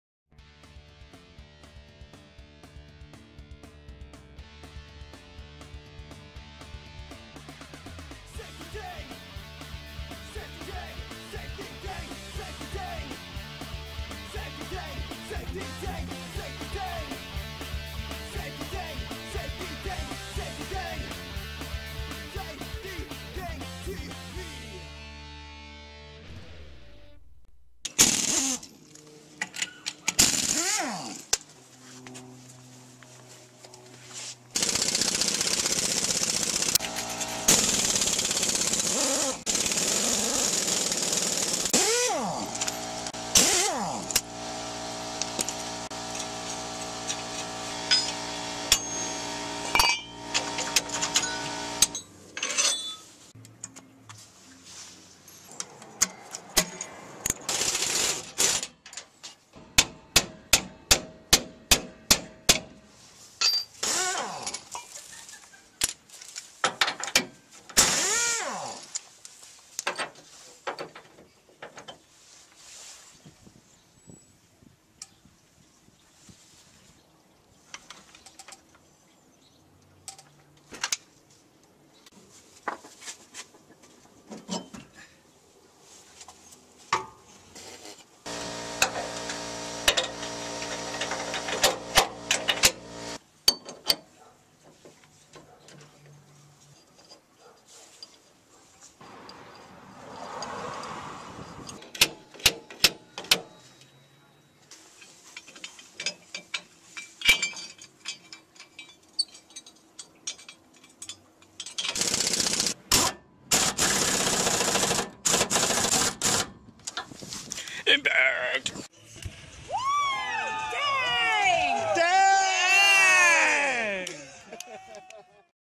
Dang Installs All Pro Expedition Leaf Springs
Dang installs All Pro  Expedition leaf springs in his 2002 Tacoma

4500T-AP-3EX-PR